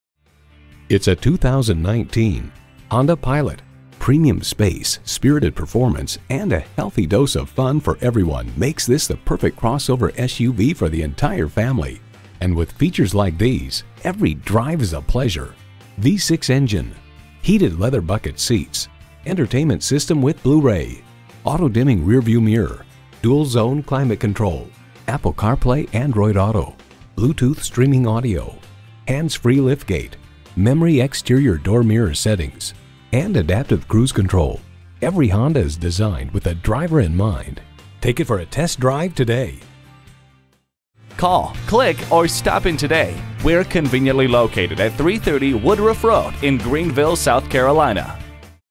Used 2019 Honda Pilot Greenville SC Easley, SC 225335A - SOLD
Year: 2019
Make: Honda
Model: Pilot
Trim: Touring
Engine: 3.5L V6 24V SOHC i-VTEC
Transmission: 9-Speed Automatic
Color: Red
Interior: Beige
Mileage: 32867
Stock #: 225335A
VIN: 5FNYF5H60KB036847
Come on out to Breakaway Honda to see this vehicle in person! CARFAX One-Owner. Clean CARFAX. Priced below KBB Fair Purchase Price!

Address:
330 Woodruff Road